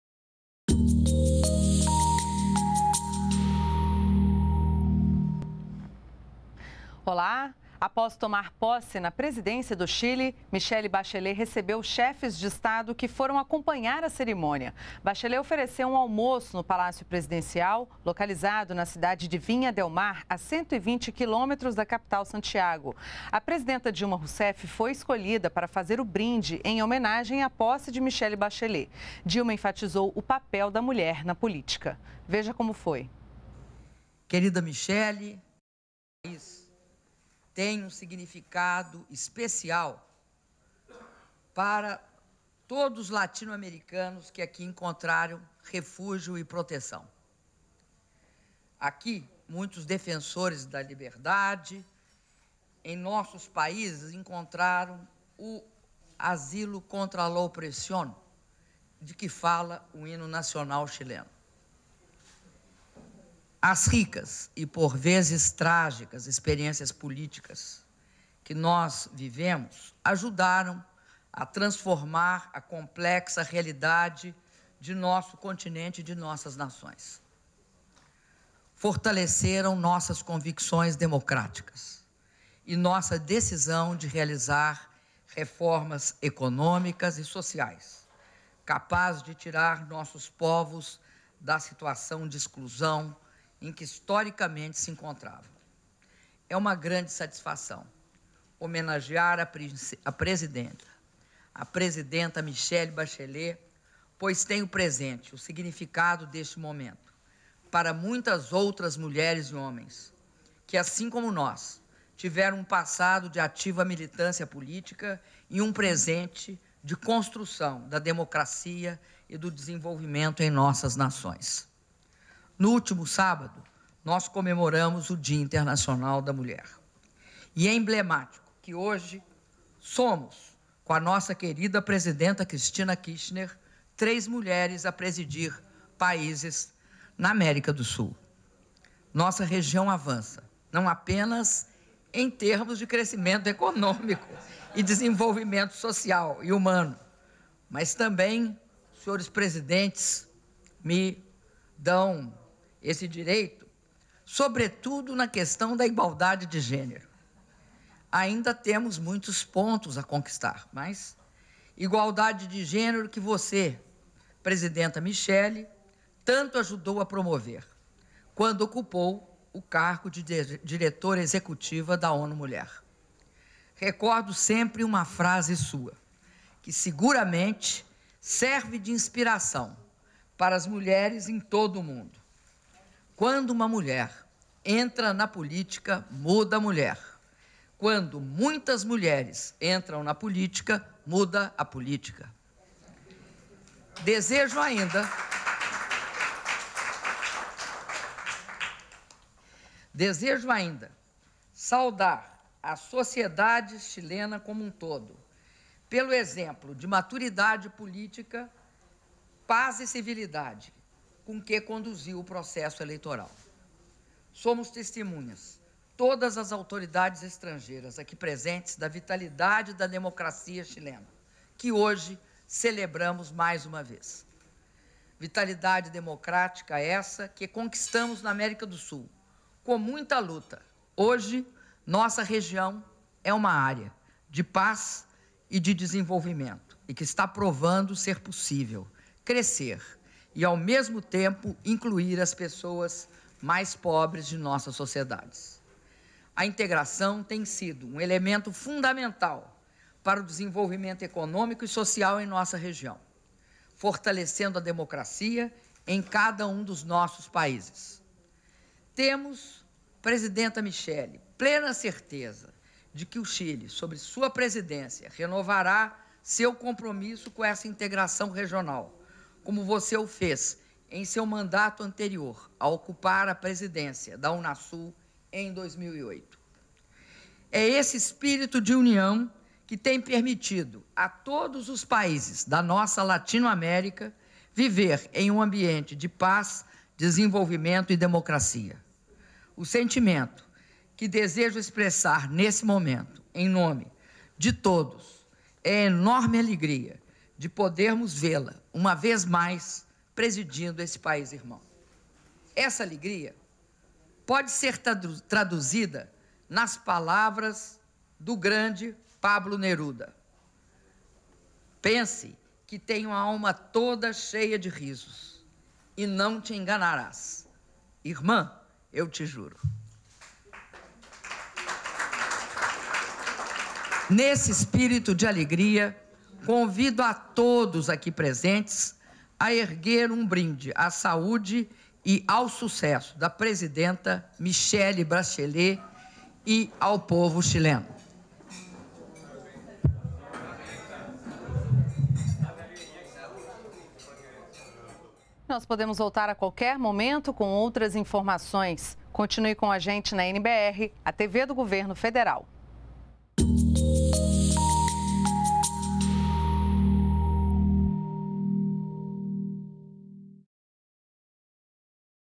Dilma Rousseff enfatiza poder da mulher na política em brinde no Chile
ATIVIDADES DA PRESIDENTA - 11.03.14: A presidenta Dilma Rousseff participou nesta terça-feira (11), em Viña Del Mar, no Chile, da cerimônia de posse de Michelle Bachelet como presidente do país. Após o evento, foi oferecido um almoço na palácio presidencial, onde Dilma Rousseff foi escolhida para conduzir o brinde em homenagem à nova chefe de Estado. Durante o discurso, Dilma enfatizou o poder da mulher na política, e lembrou que cada vez mais países da América do Sul são liderados por mulheres, algo fundamental para estimular a igualdade de gênero na região.